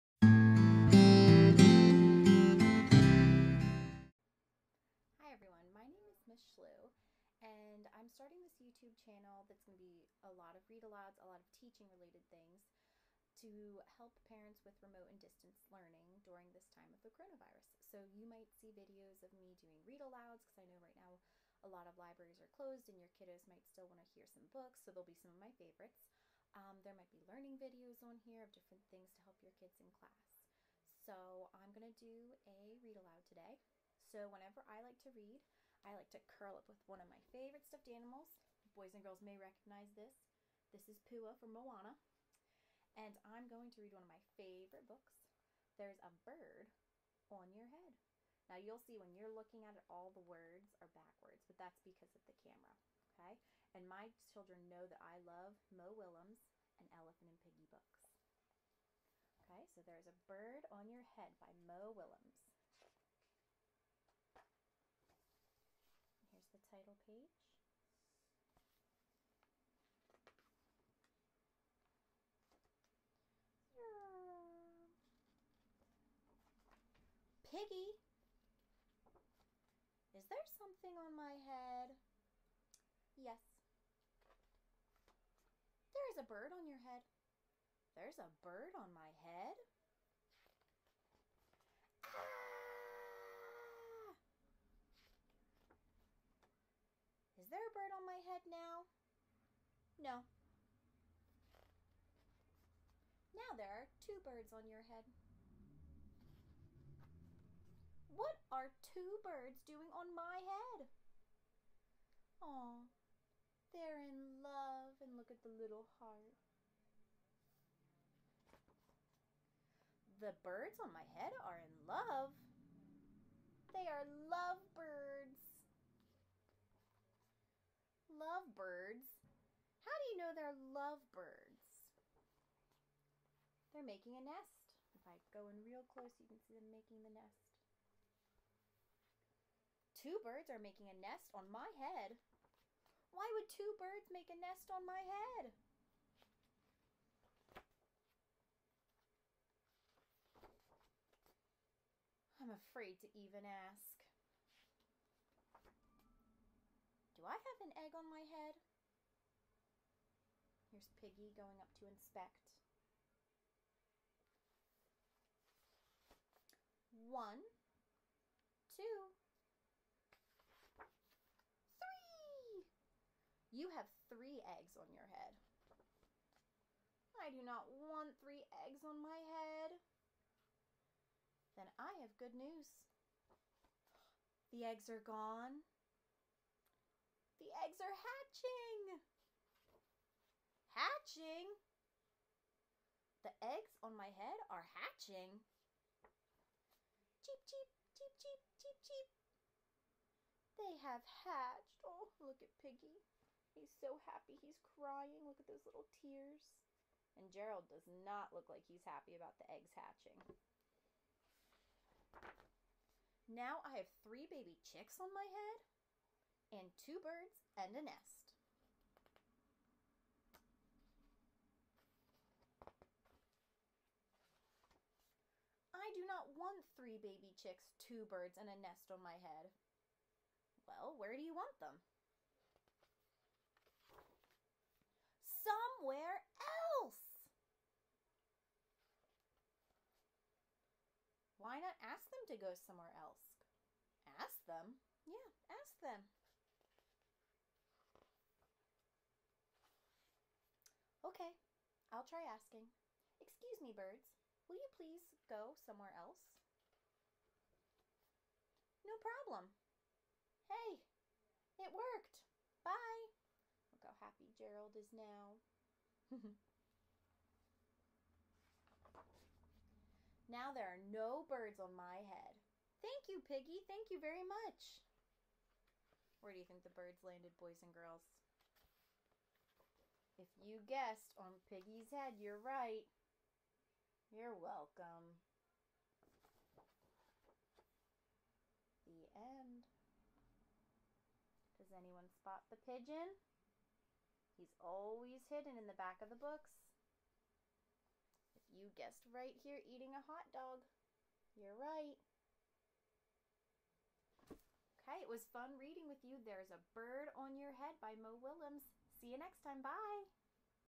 "There Is a Bird on Your Head!" By Mo Willems Read Aloud
Enjoy this read aloud of "There Is a Bird on Your Head!" by Mo Willems.